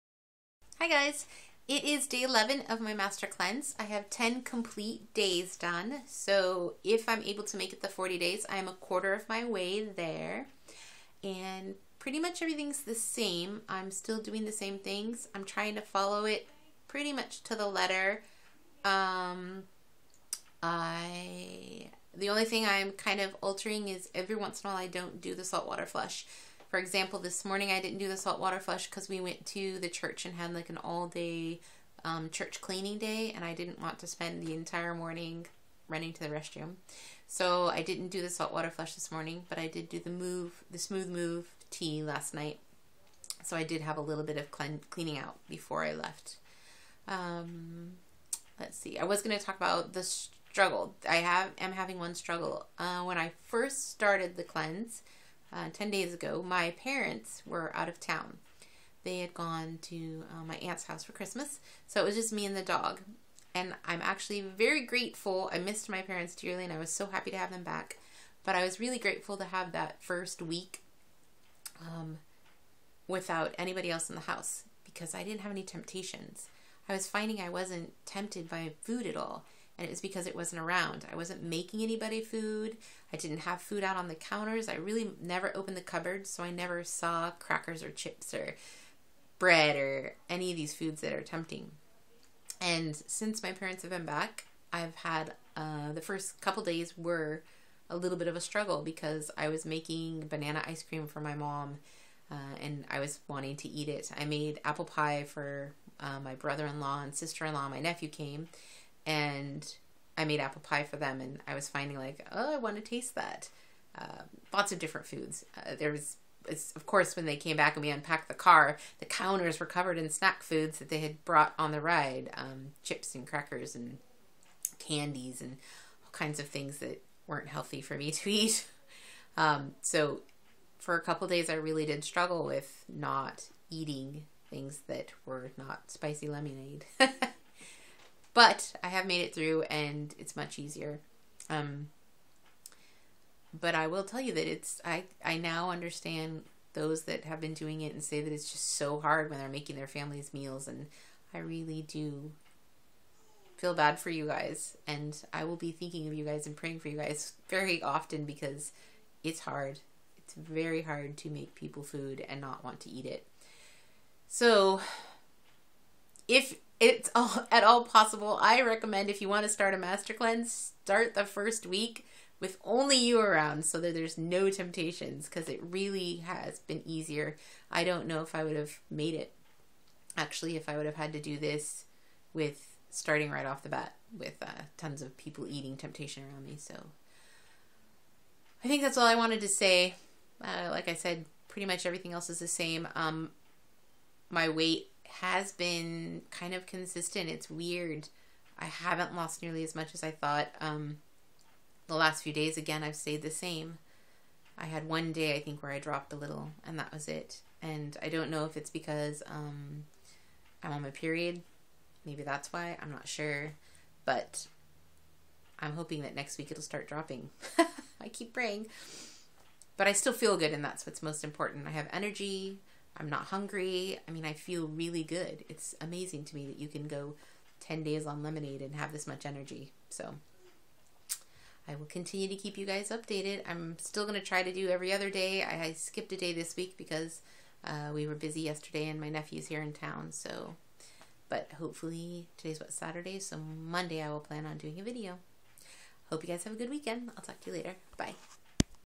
Days 8, 9 & 10 MC Complete
Parents came home and that brought some temptation. But I stood firm and I am still going strong!!!